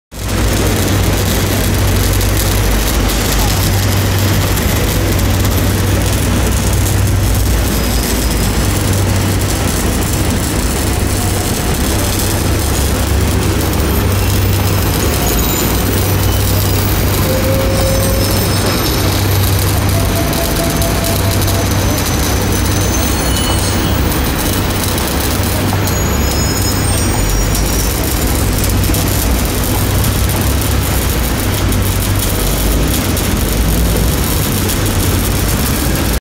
{TRAM} The Work car of HK Tramways #300 @ Causeway Bay Termini
Due to Causeway Bay Termini has track maintenance works, the work car #300 stay at Causeway Bay Termini during the track maintenance works happen until the work finish.

Tram no. : 300
Route : Causeway Bay - Whitty Street Depot
               ( After the work finish )

Place : Causeway Bay Termini
Video by : ERL62train
Camera : LG Google Nexus 5
Record Date : 14/04/2014